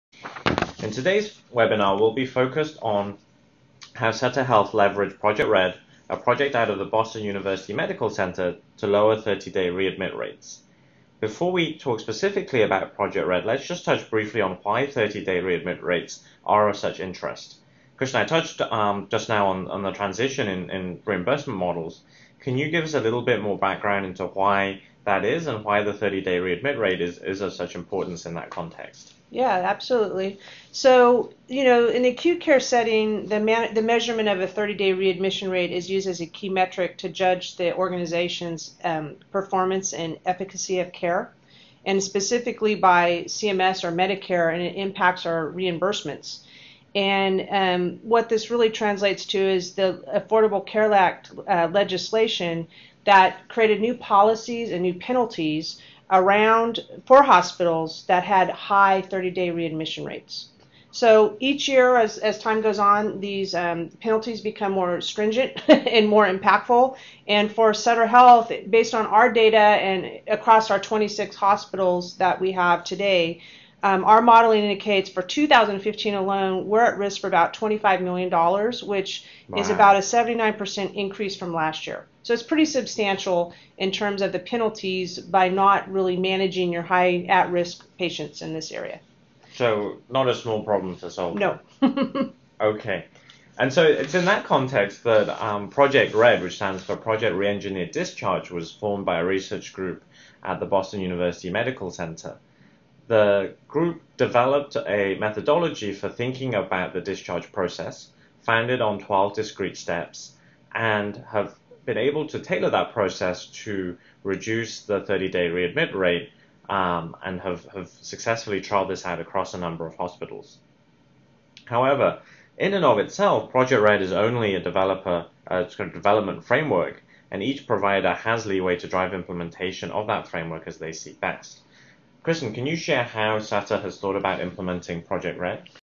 Sutter Health: Leveraging Predictive Analytics to Lower 30-Day Readmit Rates
Join us as Kristen Wilson-Jones, Sutter RD&D CTO, shares how Sutter Health has leveraged MuleSoft’s Anypoint Platform to enable real-time patient risk scoring, clinical workflow management and bi-directional integration with Epic.